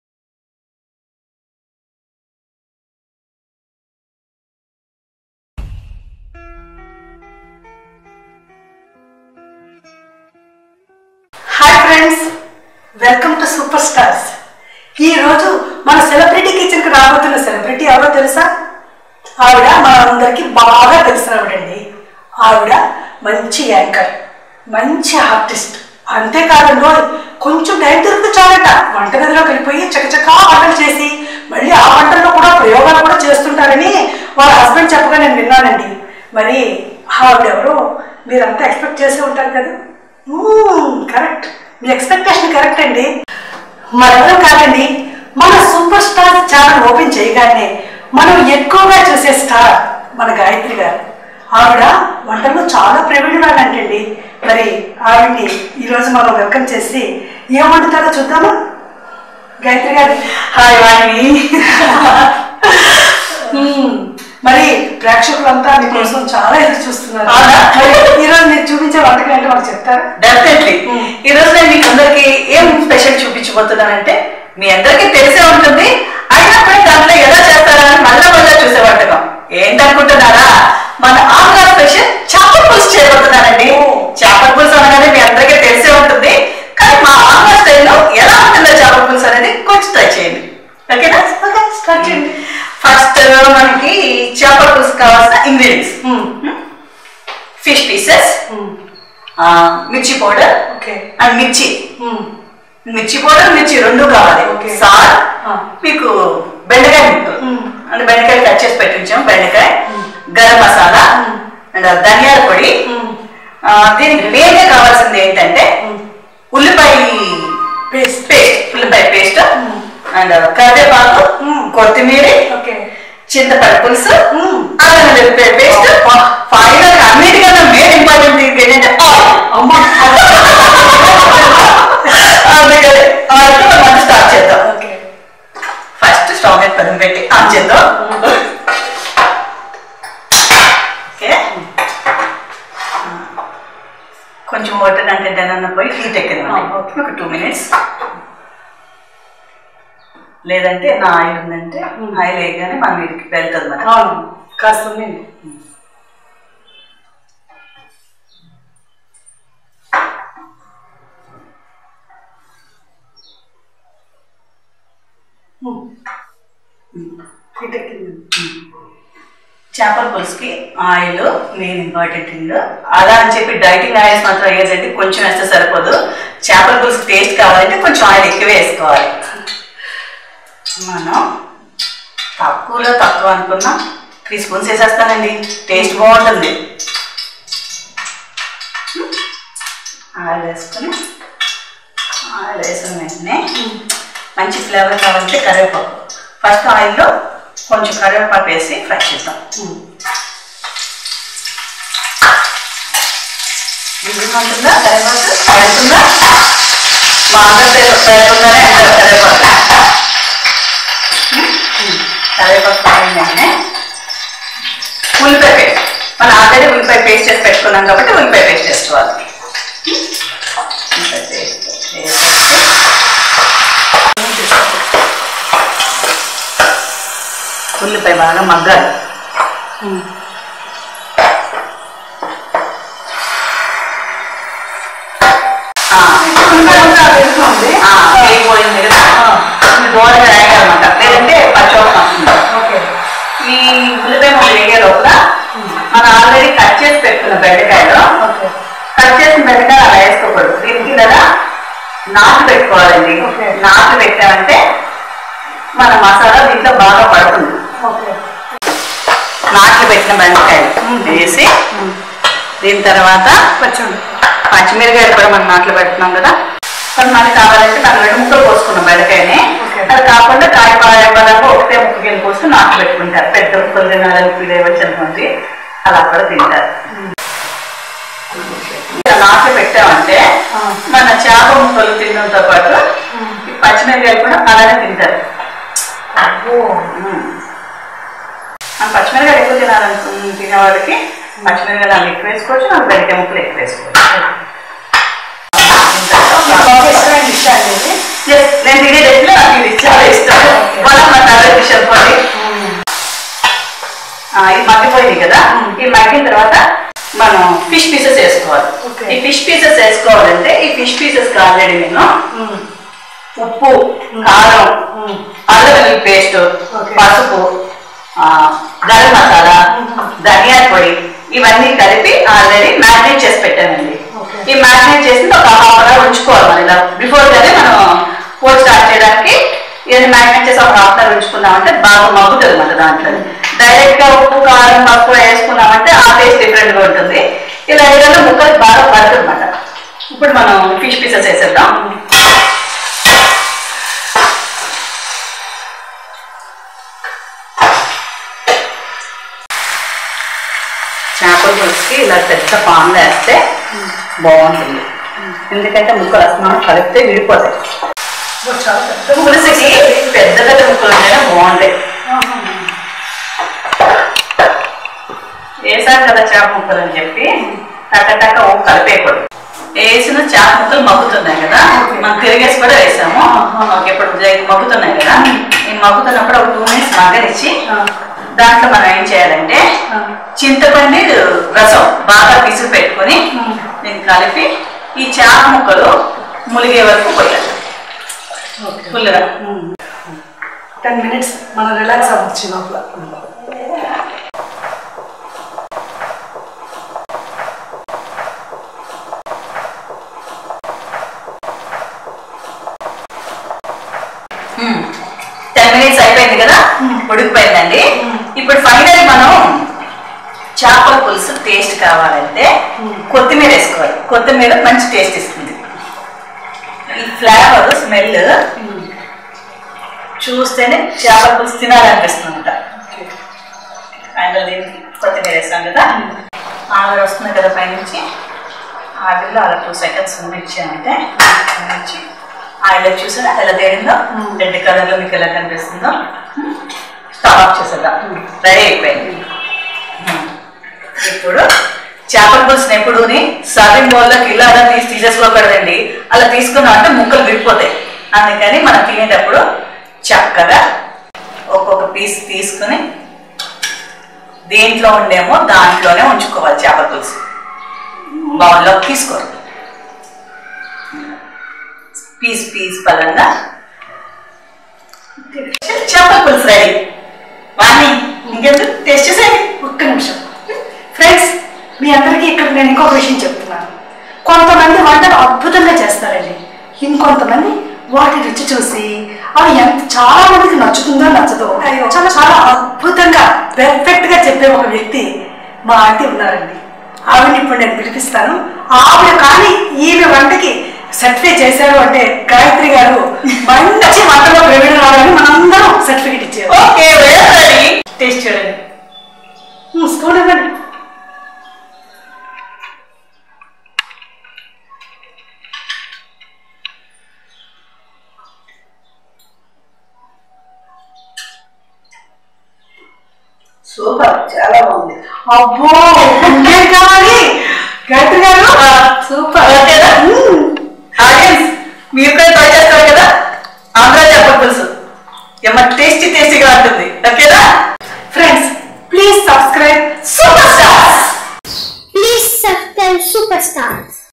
Andhra fish curry |Celebrity Kitchen with Actress gayathri & vahini || super starz||food vloge
Celebrity Kitchen with Actress gayathri & vahine || Andhra fish curry | Chepala Pulusu recipe | andhra style fish pulusu || super starz ||